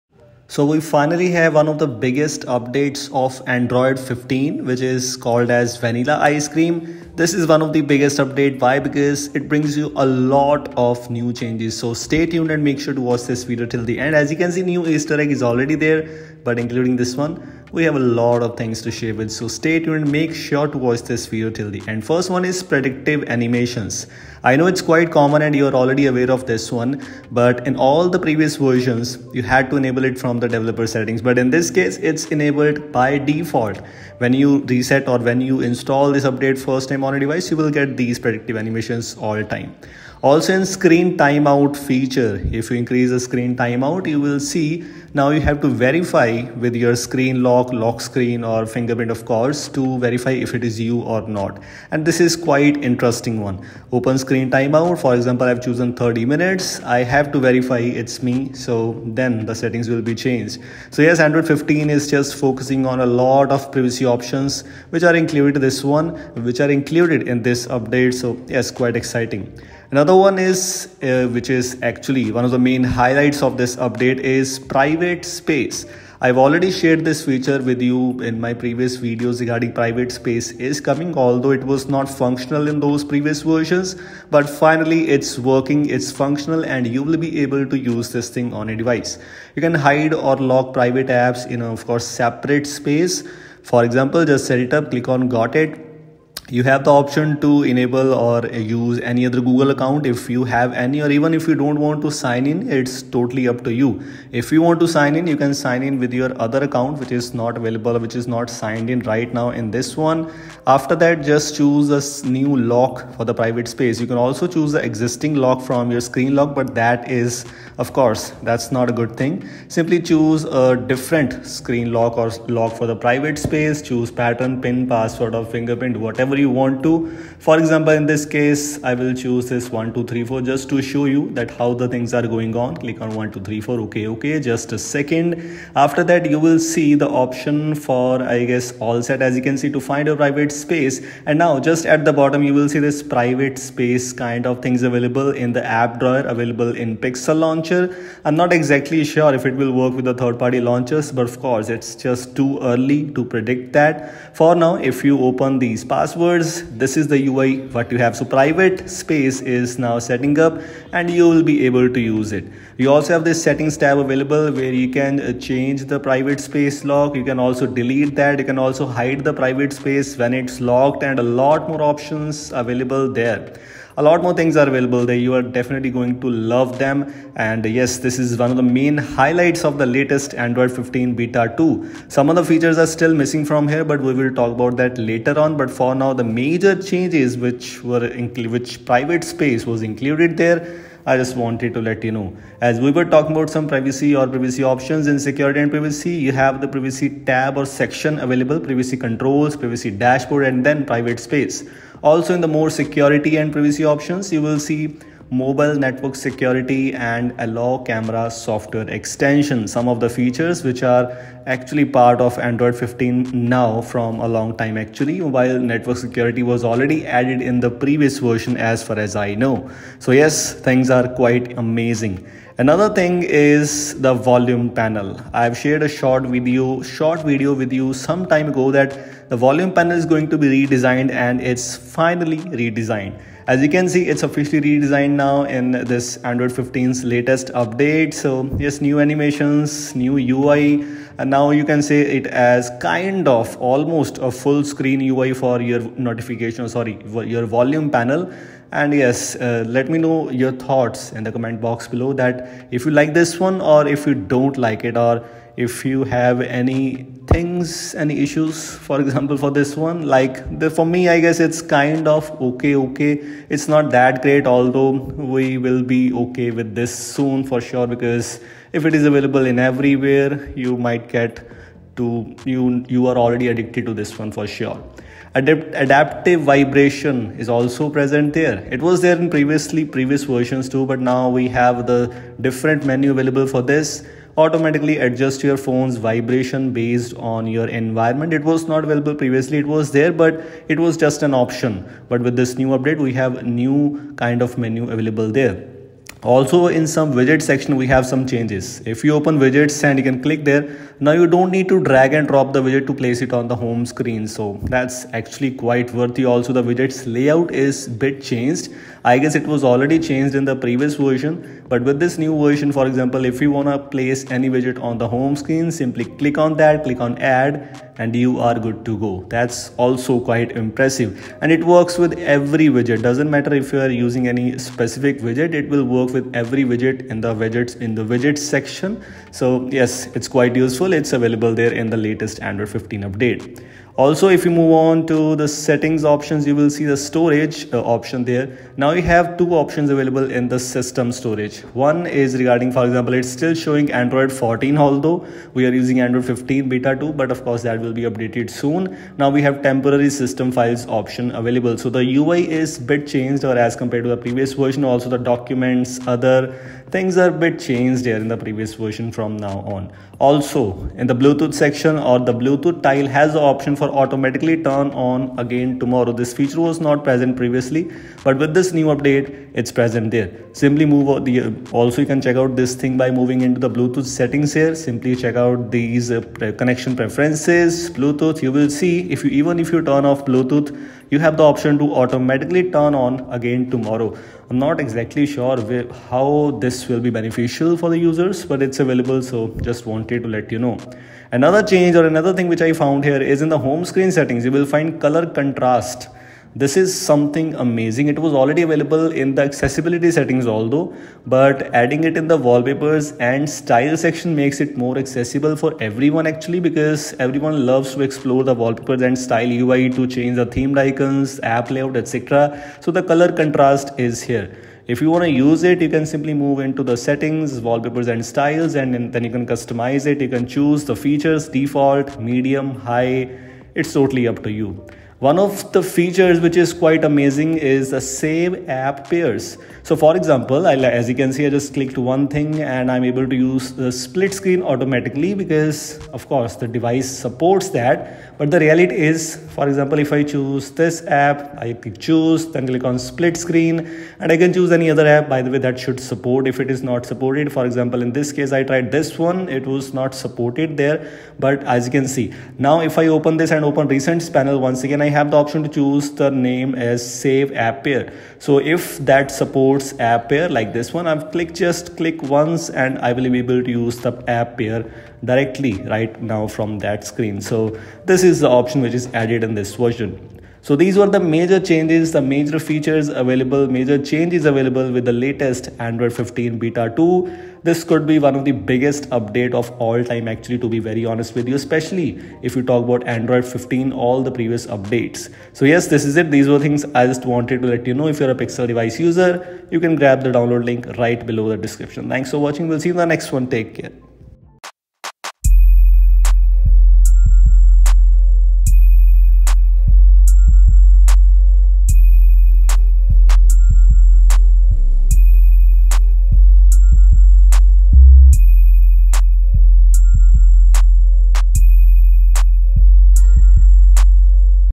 Android 15 Beta 2 Released: Just Wow! 🤯🤯
Hey guys, What's Up? Everything is good I Hope. I will show you the " Android 15 Beta 2 Review "in this video. Make Sure to watch this video till the end to understand everything.

Timeline:
00:00 Intro
00:20 Predictive Back Animations
00:36 Screen Timeout Privacy
01:08 Private Space
03:10 Privacy
03:41 Volume Panel
04:31 Adaptive Vibrations
04:51 Widgets
05:31 Storage Manager
05:56 Auto Bluetooth ON
06:28 Colour Contrast
07:03 App Pair